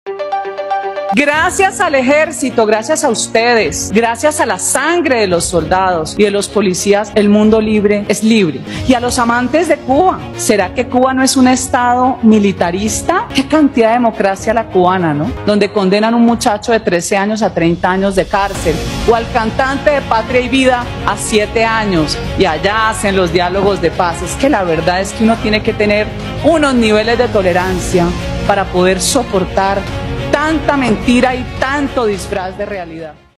¡LLAMADO A LA REFLEXIÓN! 🚀Senadora envía “contundente” mensaje a los amantes de Cuba
"Y a los amantes de Cuba: ¿será que Cuba no es un Estado militarista? ¡Qué cantidad de democracia la cubana! La verdad, uno debe tener unos niveles de tolerancia para poder soportar tanta mentira y tanto disfraz de realidad "

María Fernanda Cabal